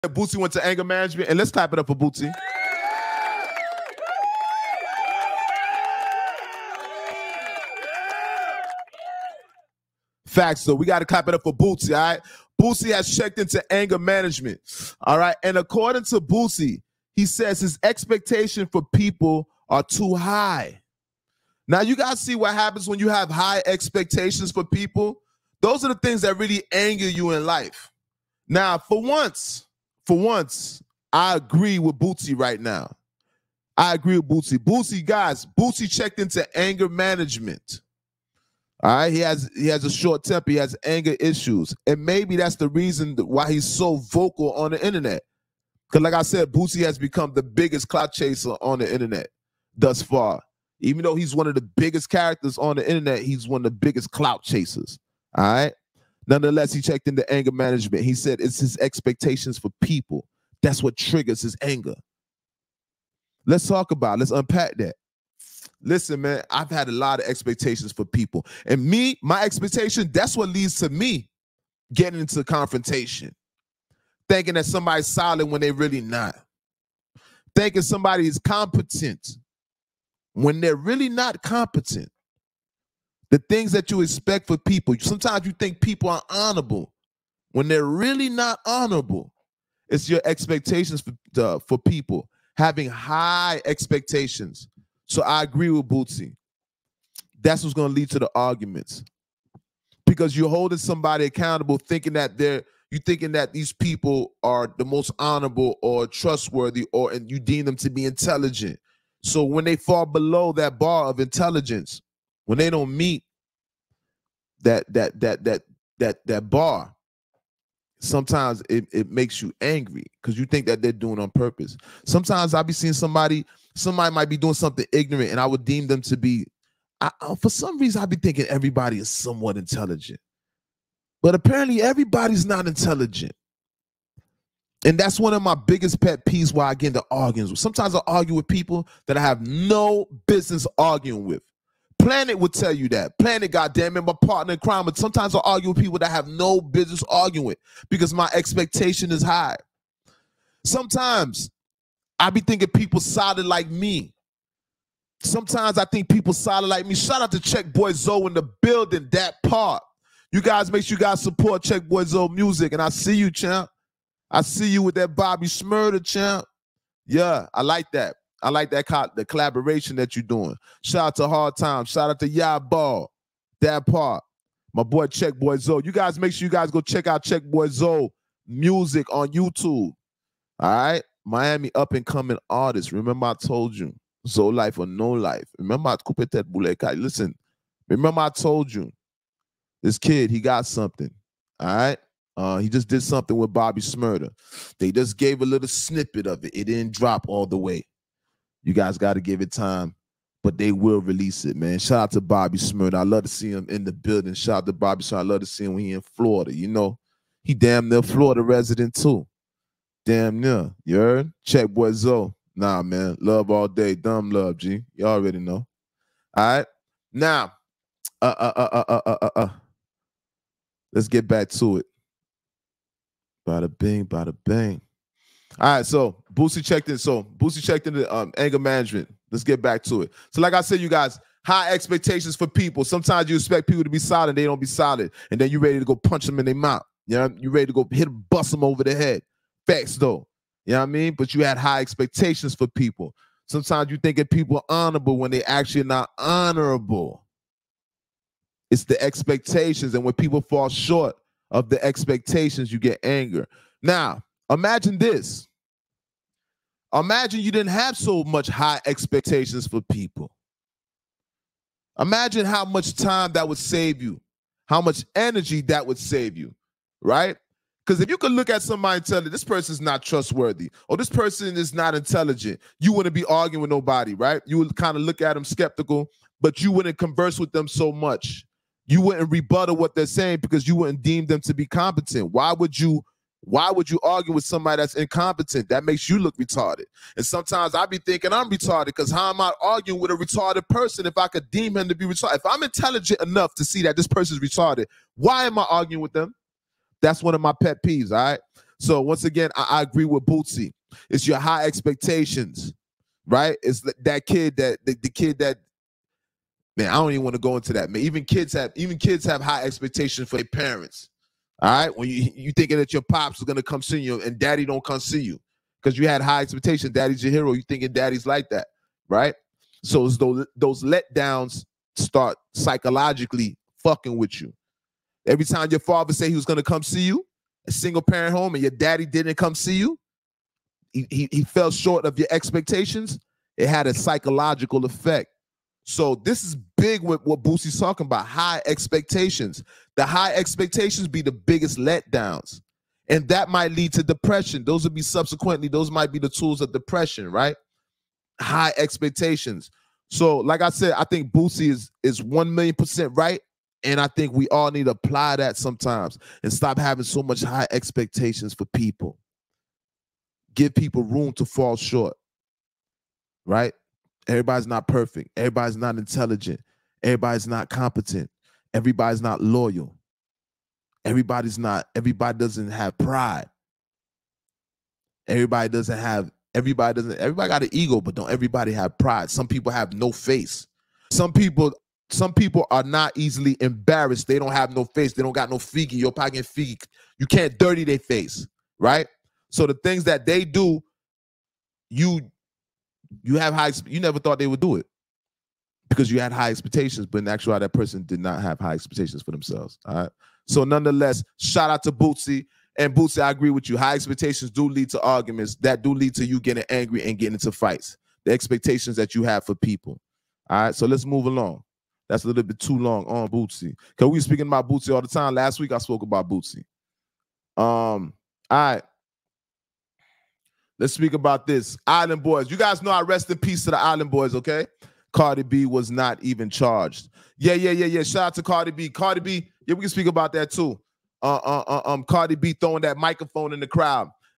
Boosie goes Anger Management says High expectations caused anger
Boosie has arrived to ￼the conclusion after doing some self reflection that he is in urgent need of anger management. Boosie took to the social network quoted, saying his high expectations for people is what leads to him becoming angered.